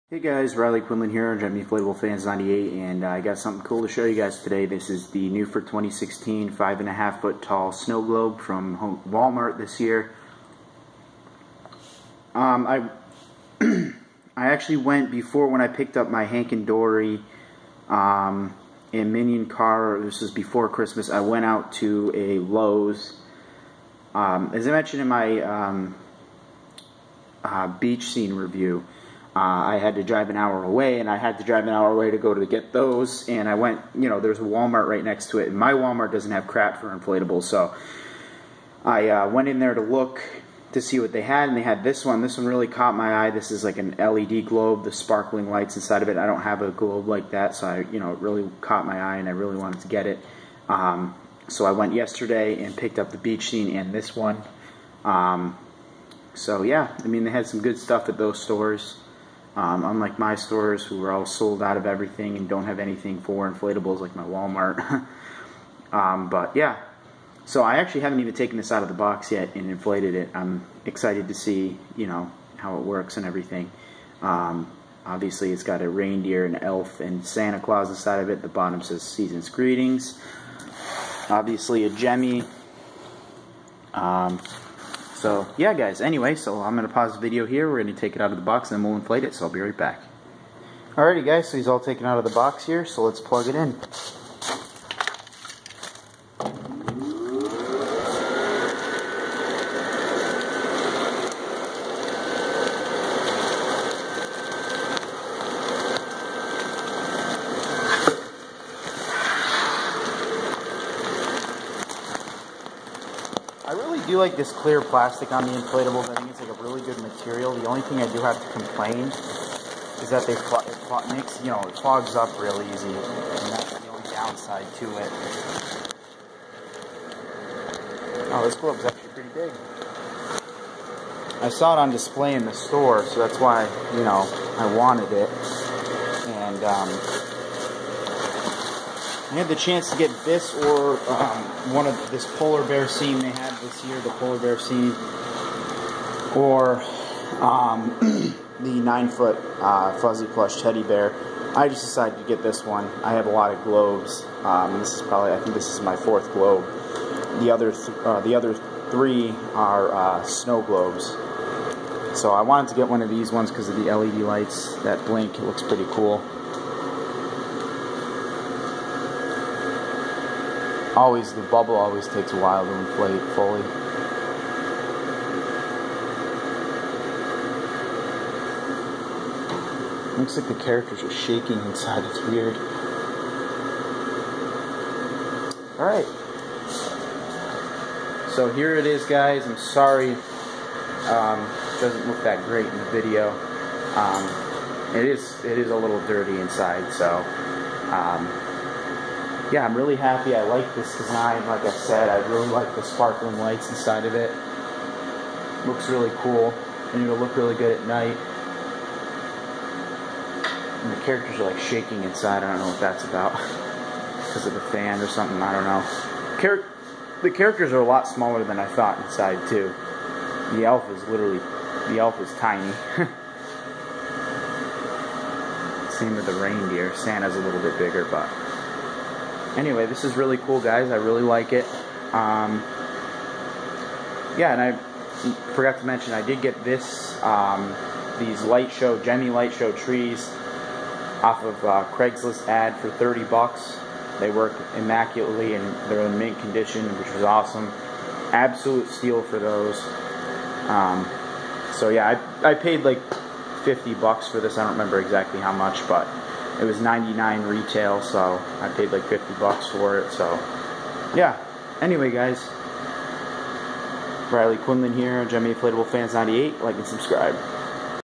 Unboxing/Review Gemmy 2016 5.5ft Snowglobe Airblown Inflatable Review
Got this awesome inflatable at a Walmart about an hour away from me! This is a really cool inflatable and i definitely recommend getting it! It has a cool LED light blinking effect inside that i found to be really cool!
Be sure to check in on this channel for daily videos!